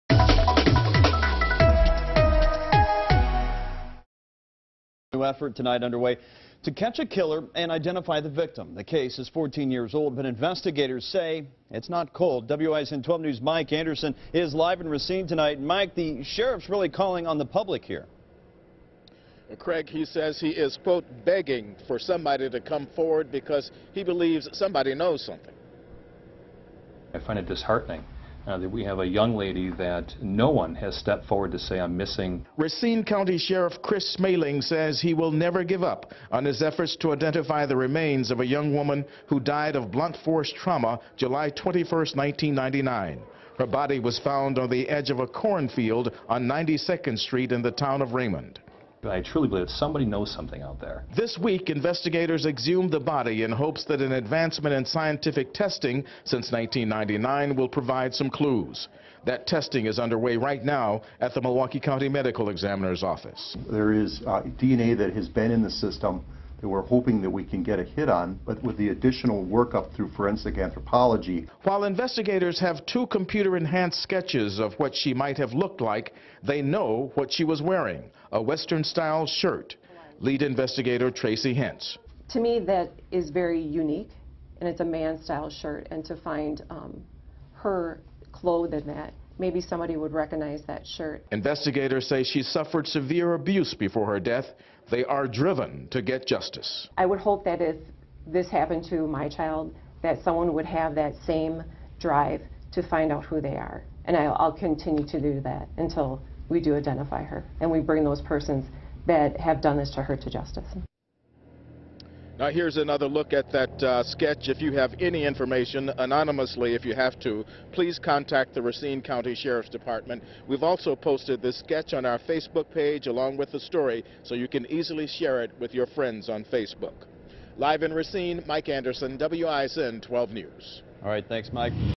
New science may help investigators catch a killer, identify victim
Racine County investigators aren't giving up hope on identifying a Jane Doe found dead 14 years ago in a field in Raymond.